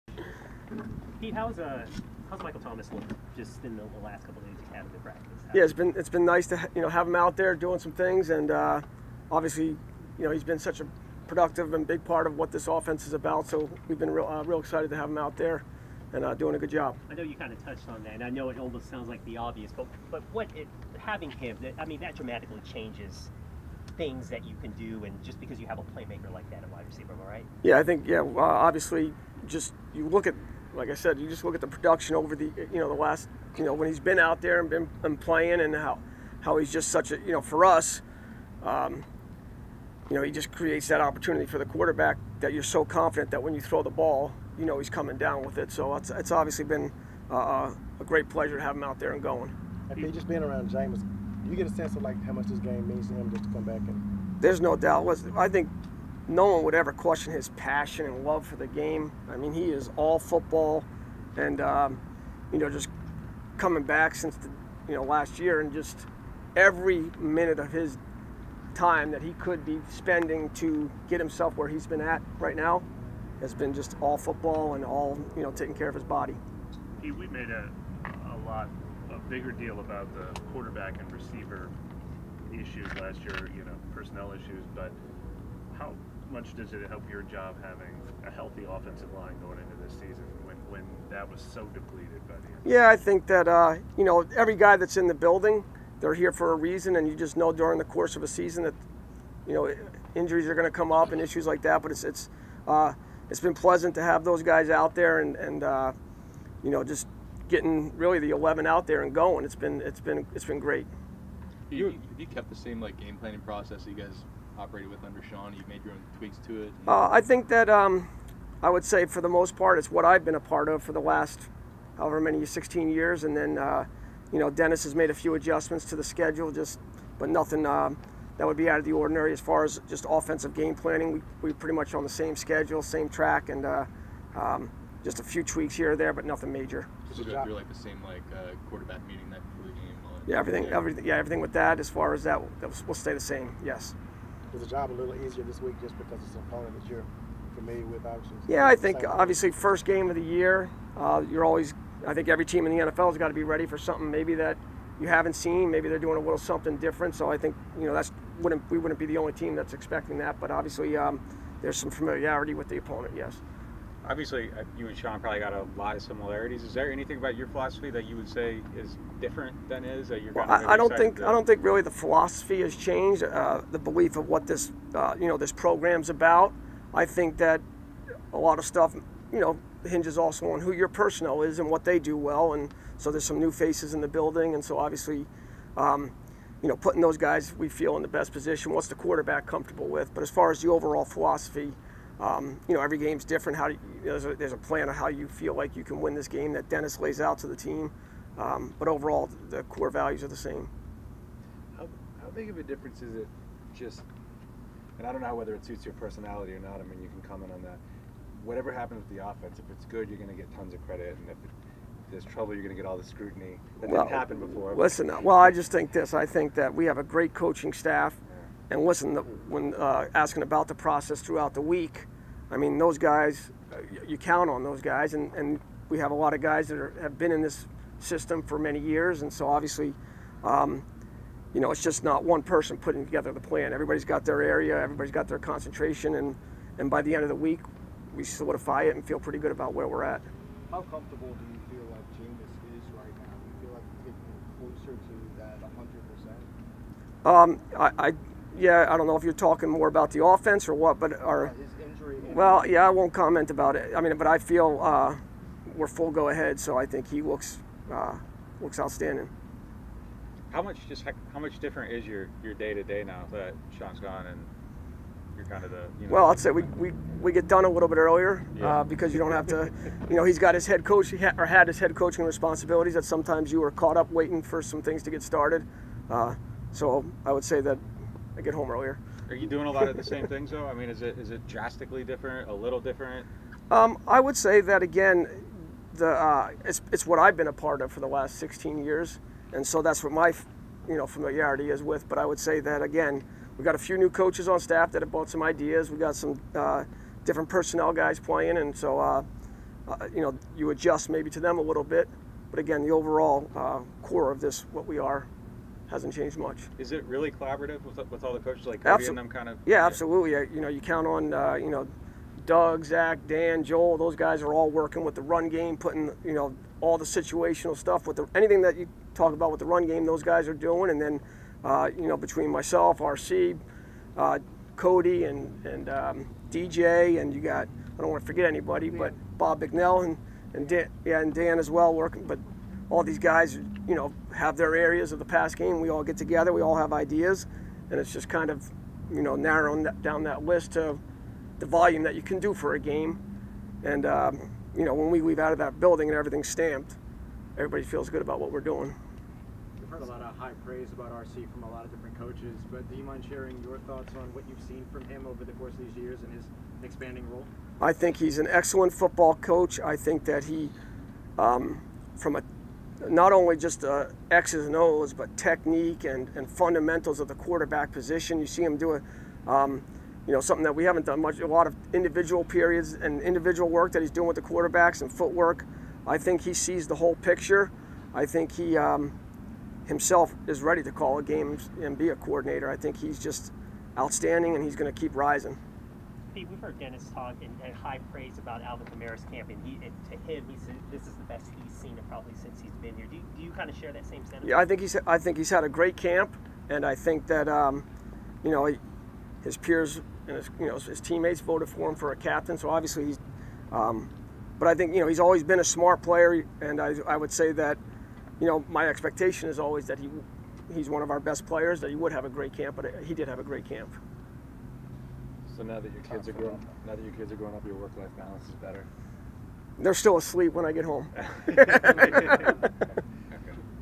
Pete Carmichael on State of the Offense Entering Week 1 | New Orleans Saints
New Orleans Saints offensive coordinator Pete Carmichael post-practice media availability on Thursday, September 8 at the Ochsner Sports Performance Center.